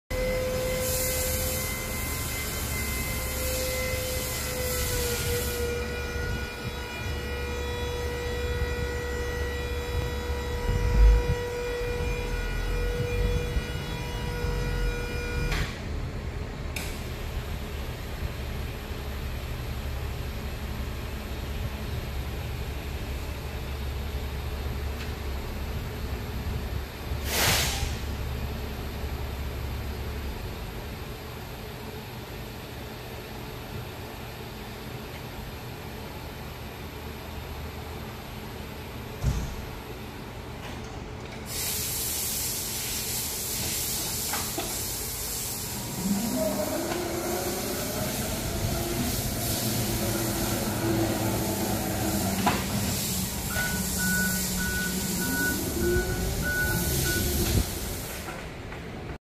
Yale 0S030EAN24TE095 order picker for sale at auction | bidding closes June 15, 2022
Yale 0S030EAN24TE095  order picker
 Hours: 3,511 on meter
 Serial: A801N06557U
Electric Motor
 Volts: 24
 Phase: Single
Features
 Maximum lift capacity: 3,000 lbs
 Maximum lift height: 213"
 Mast stages: 3
 Fork length: 42"
Tires
 Solid
It's located in Grain Valley, MO, and we're selling it to the highest bidder regardless of price.